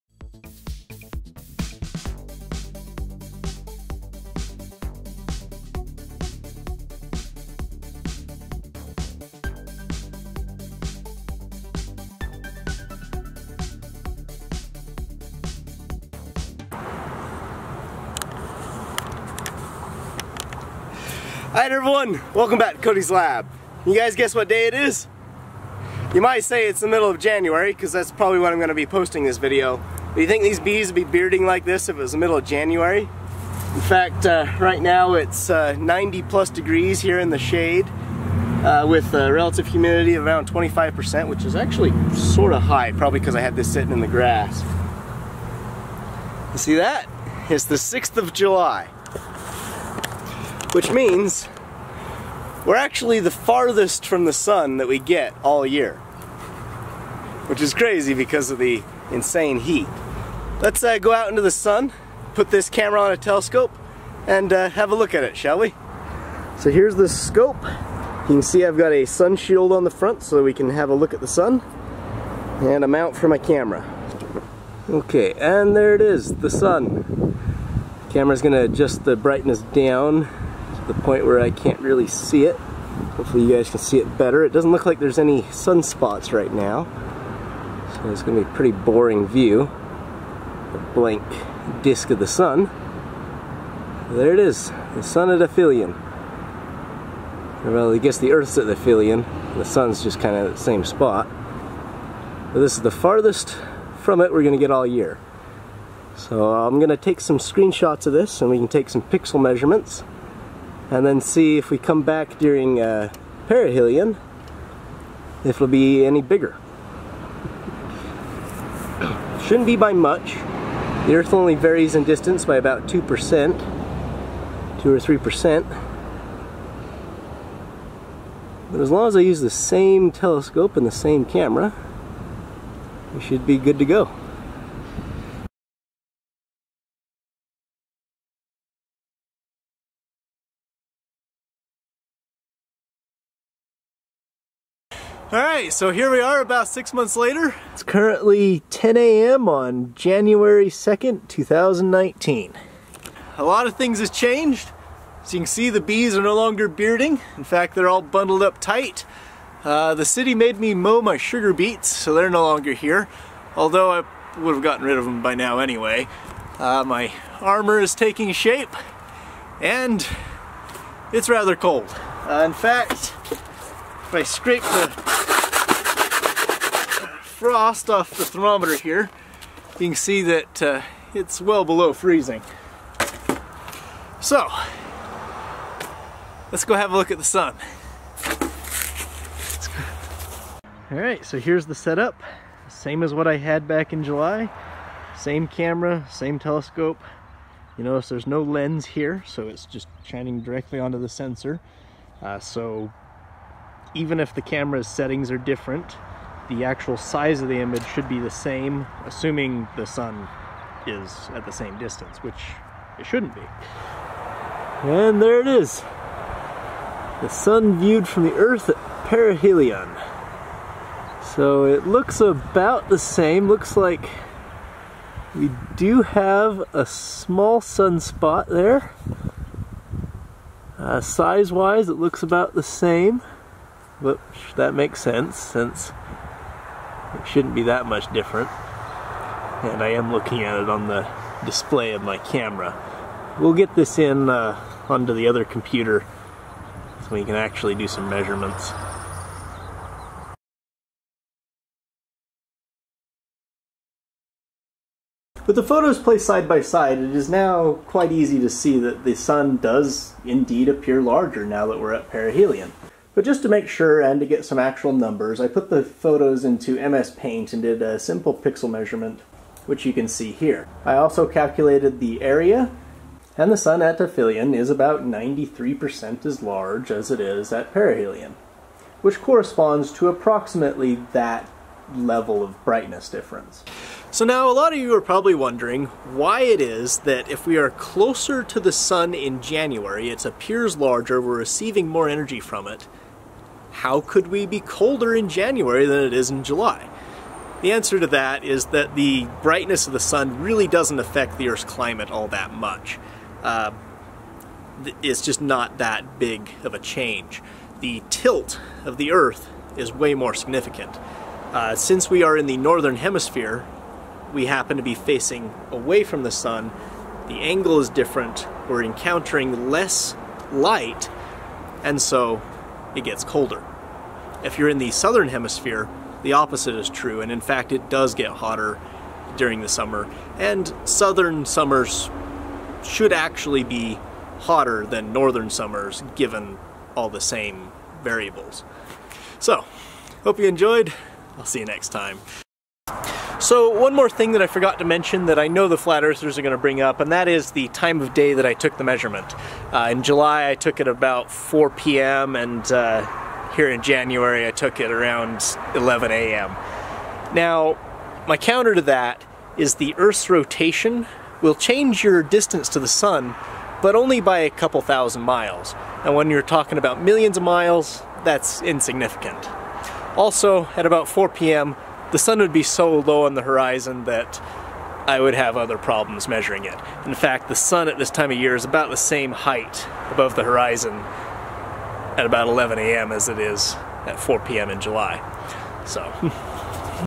Sun at Aphelion Vs Perihelion
I show how the sun visibly changes in apparent size throughout the year.